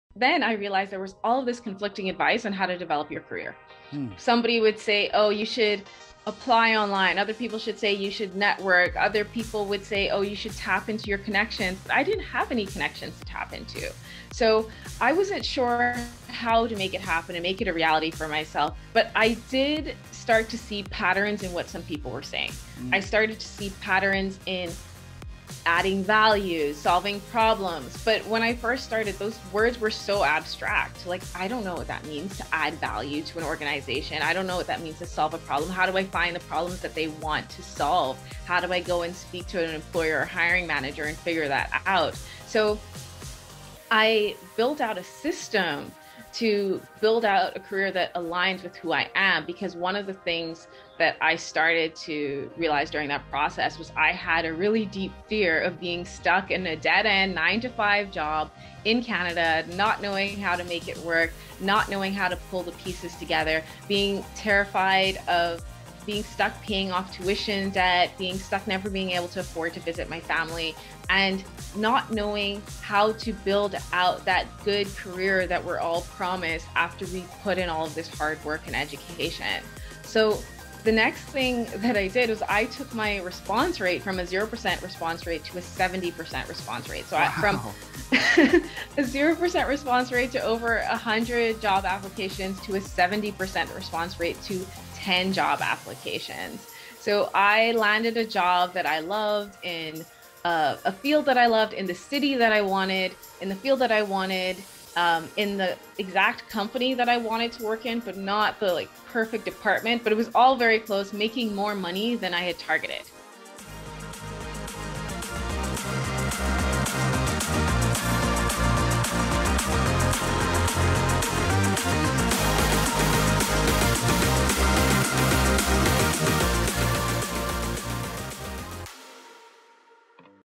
Taking Your Job Application Response Rate from 0% to 70% with Jette Stubbs
Brandon and Jette Stubbs talk about how to make a job change when you don't have a huge network and don't know what it means to "add value" to an employer.